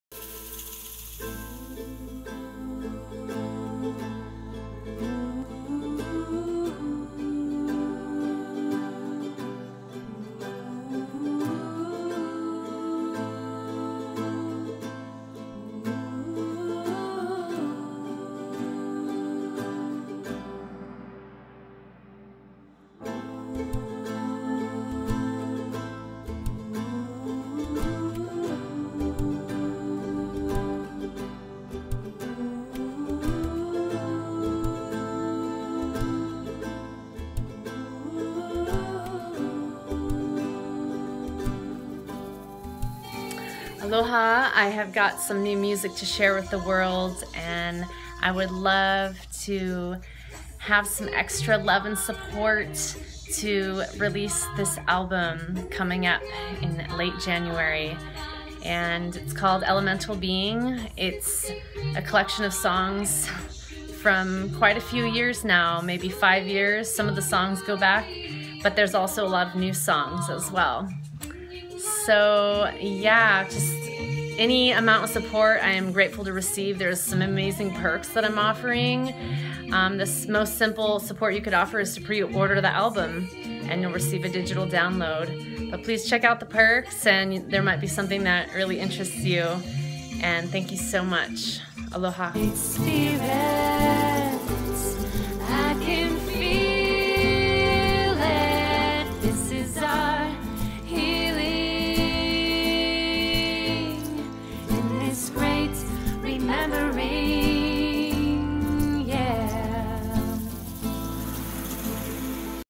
INDIEGOGO-for my new Album launch!
This is the little video I made for my INDIEGOGO campaign I have launched , asking for some support in getting my Music OUT THERE to the world! You can PRE-Order my new album , to be released in late January and there's many other awesome perks I am offering, so check it out!!